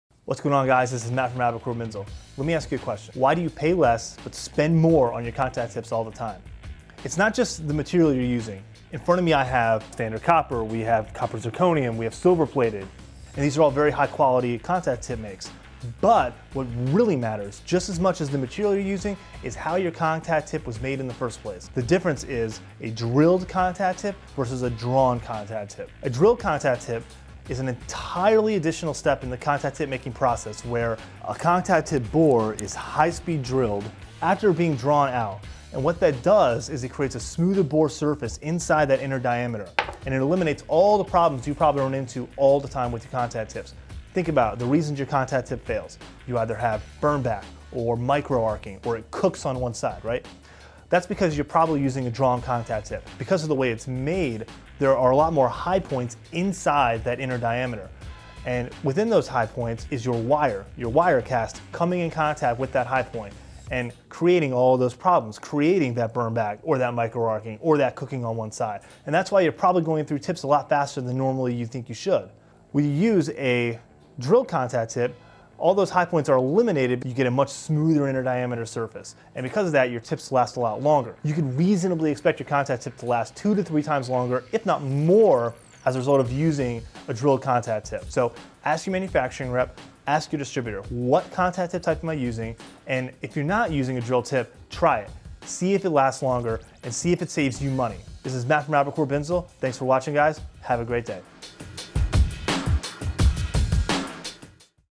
Drilled vs Drawn Contact Tips | Binzel Basics
The process of how a contact tip is built has an enormous impact on its longevity. Using drilled contact tips could improve your weld and reduce cost!